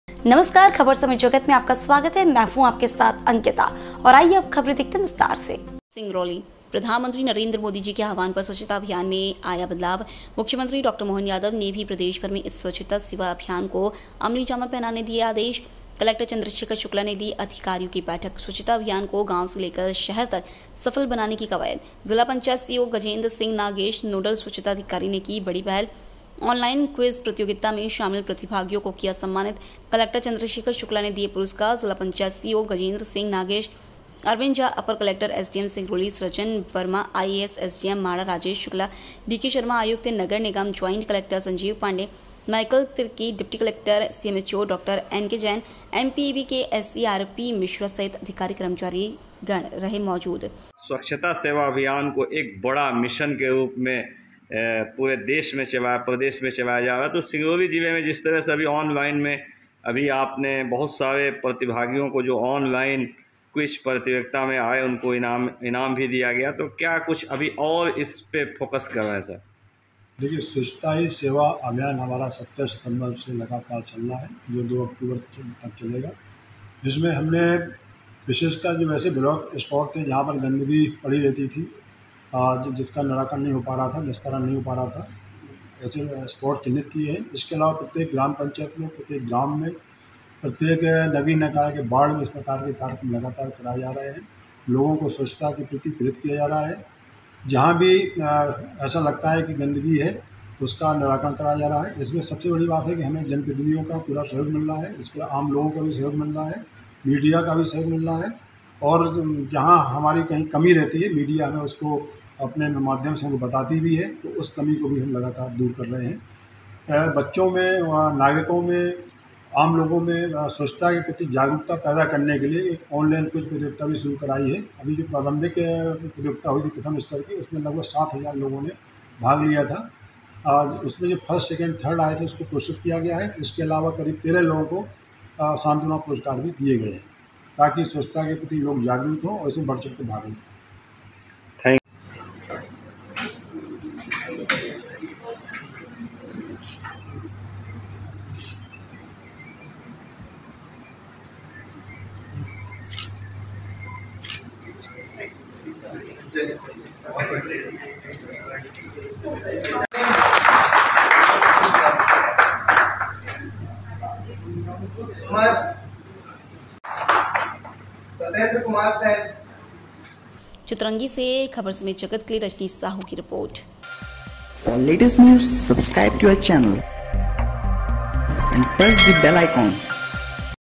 चितरंगी:- प्रधानमंत्री नरेंद्र मोदी जी के आह्वान पर स्वच्छता अभियान में आया बदलाव !
Reporter -  रजनीश साहू